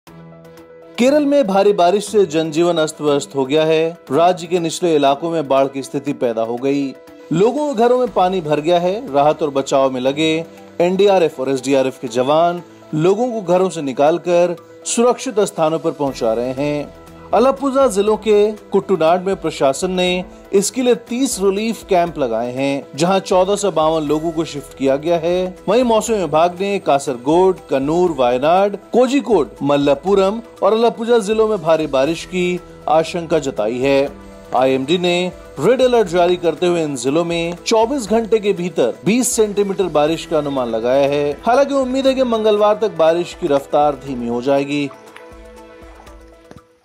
केरल में बाढ़ के हालात, भारी बारिश का रेड अलर्ट जारी
केरल में भारी बारिश से जनजीवन अस्त-व्यस्त हो गया है. राज्य के निचले इलाकों में बाढ़ की स्थिति पैदा हो गई. लोगों के घरों में पानी भर गया है. राहत और बचाव में लगे NDRF और SDRF के जवान लोगों को घरों से निकाल कर सुरक्षित स्थानों पर पहुंचा रहे हैं. अलप्पुझा जिलों के कुट्टनाड में प्रशासन ने इसके लिए 30 रिलीफ कैंप लगाए हैं जहां 1452 लोगों को शिफ्ट किया गया है. वहीं मौसम विभाग ने कासरगोड, कन्नूर, वायनाड, कोझिकोड, मलप्पुरम व अलप्पुझा जिलों में भारी बारिश की आशंका जताई है. आइएमडी ने रेड अलर्ट जारी करते हुए इन जिलों में 24 घंटे के भीतर 20 सेंटीमीटर बारिश का अनुमान लगाया है. हालांकि उम्मीद है कि मंगलवार तक बारिश की रफ्तार धीमी हो जाएगी.